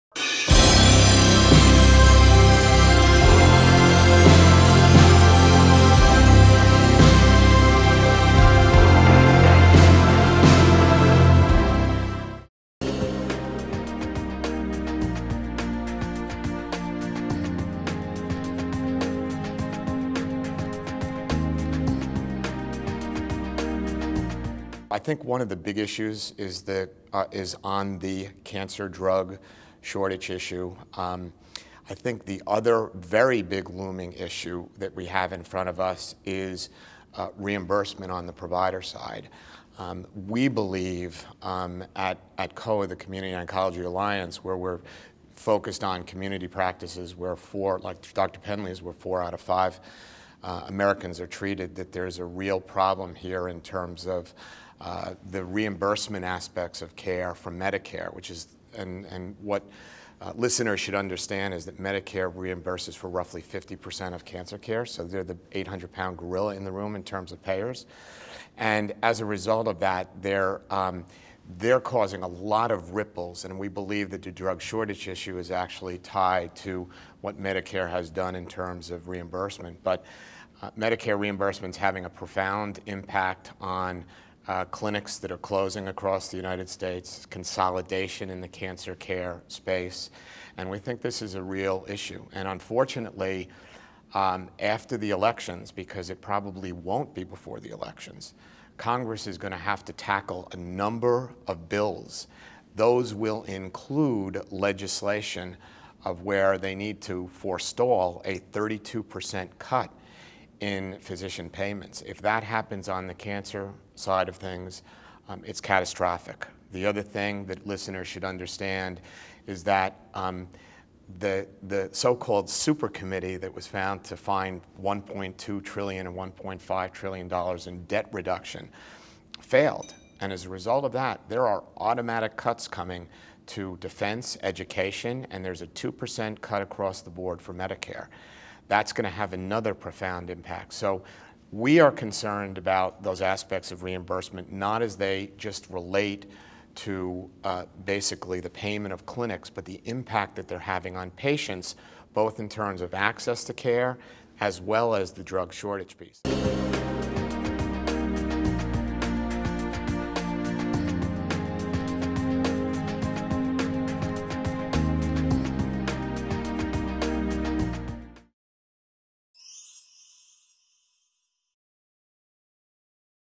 Advocacy in Action: Community Oncology Alliance (COA) Public Policy Initiatives
In this Advocacy in Action episode, Ted Okon (Executive Director, Community Oncology Alliance - COA) talks about the public policy focus for the Community Oncology Alliance.

Public policies may encourage or prohibit further advancements or access to the latest care and treatments. Although cancer survivors are more likely to return to their jobs and families, they are forever changed by their experiences. Legislators may have a different view the role of public policies after they consider the issues of cancer survivors and their families.

There are many legislative and regulatory issues that surround Public Policy Advocacy and impact oncology care and that influence clinical advancements and access to treatment.

Some of the issues in cancer public policy include:
• Drug Shortages
• Advanced Cancer Care Planning
• NIH/NCI/FDA Appropriations
• Access to Care
• Value in Cancer Care
• Drug Safety
• Reimbursement
• Medicare

Joining Vital Options CEO, Selma Schimmel, as co-moderators for Public Policy Advocacy in Cancer Care will be Nancy Davenport-Ennis, Patient Advocate Foundation; Linda House, Cancer Support Community; Ted Okon, Community Oncology Alliance; Charles Penley, MD, ASCO Government Relations Committee; and Scott Williams, Men's Health Network.